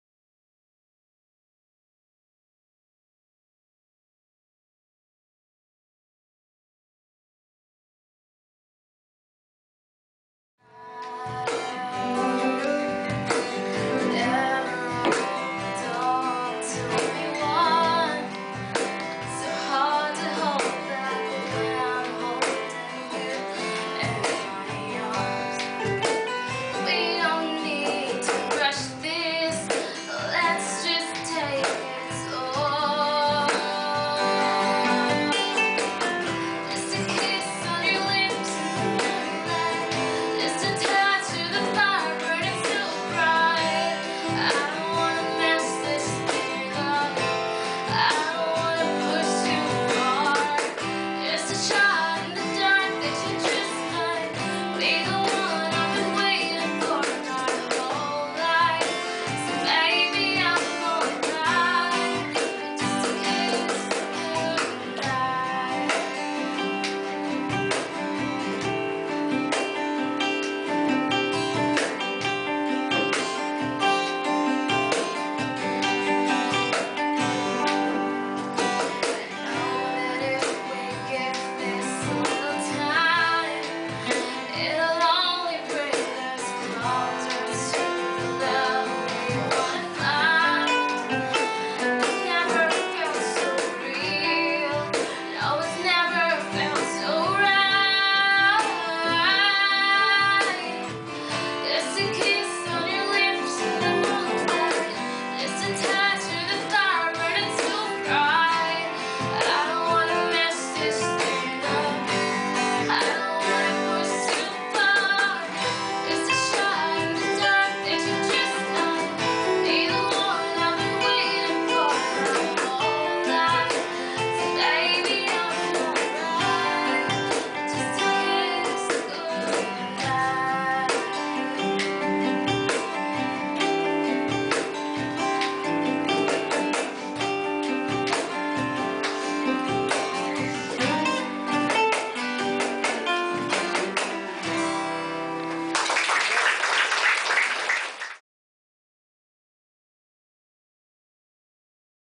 Max Clouth Jams With Swadha
Max Clouth Jamming with Swadha, a TMG student at the Jazz and Flamenco Workshop held at The Music Gurukul.